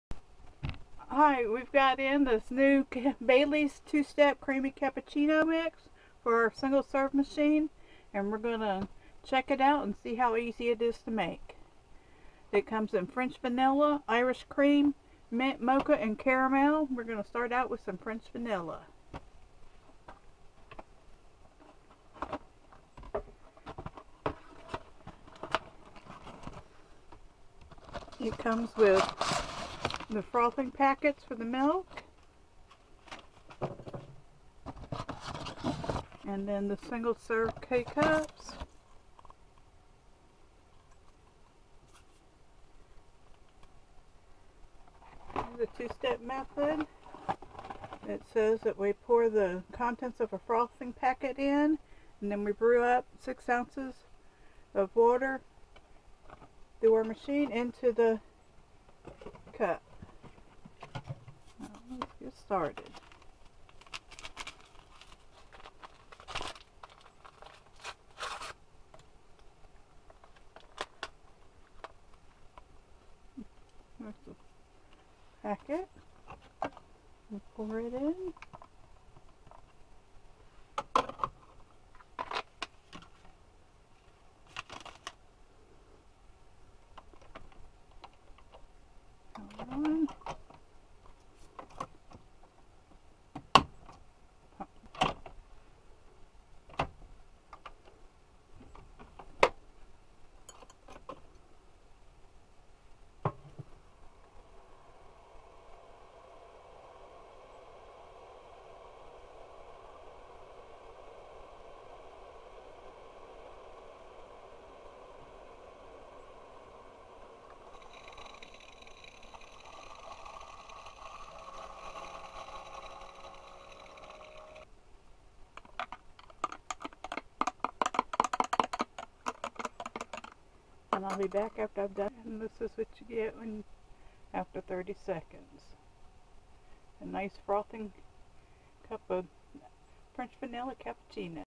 Be Your Own Barista With Baileys Cappuccino For Keurig Single Serve Machines
Rich, decadent flavors French Vanilla, Caramel Latte, Mint Chocolate and Irish Cream flavor combined with a smooth coffee taste topped off with creamy foam.

Now, your Keurig machine is a coffee house and you are the barista with these Baileys 2 step Cappuccino kits. Simply pour the powder packet of frothing milk into your mug or glass and brew the single serve coffee cup. Then stir and watch the creamy, dreamy magic happen. It’s a perfectly flavored cappuccino every time, no foaming, frothing, or tipping required.